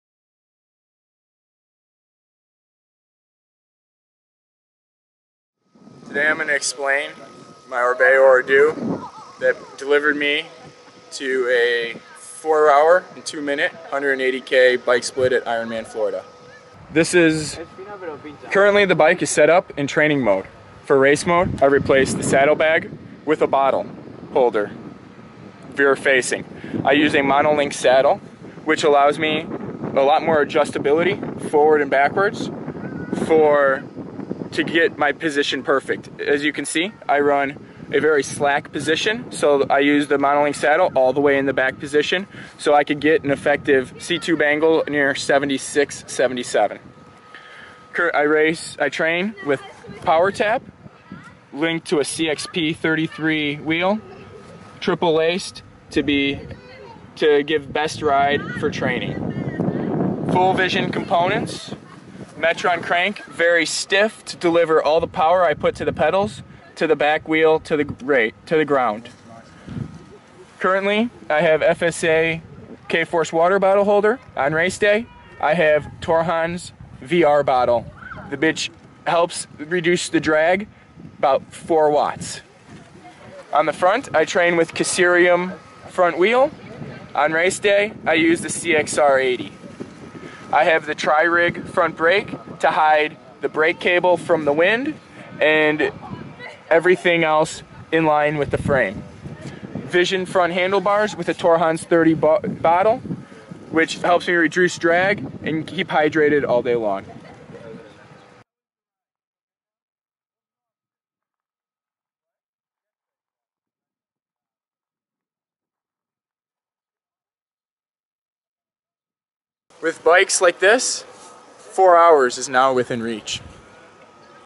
Orbea Ordu original WR IM by Starykowicz
Andrew Staryckowicz explica la bici con la que obstenta el Record del Mundo de IM